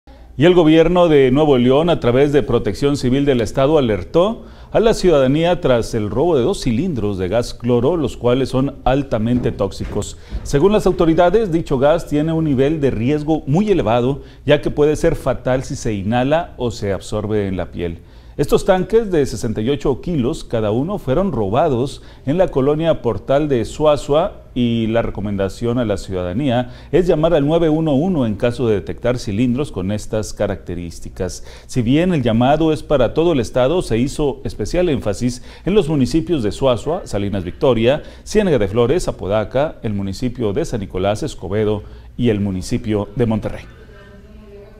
Alertan por robo de cilindros de gas cloro altamente peligrosos
El Gobierno de Nuevo León, a través de Protección Civil del Estado, alertó a la ciudadanía tras el robo de dos cilindros de gas cloro, los cuales son altamente tóxicos.
Según las autoridades, dicho gas tiene un nivel de riesgo elevado, ya que puede ser fatal si se inhala o se absorbe en la piel.
Los tanques, de 68 kilogramos cada uno, fueron robados en la colonia Portal de Zuazua y la recomendación a la ciudadanía es llamar al 911 en caso de detectar cilindros con éstas características.
Si bien el llamado es para todo el estado, se hizo especial énfasis en los municipios de Zuazua, Salinas Victoria, Ciénega de Flores, Apodaca, San Nicolás, Escobedo y Monterrey.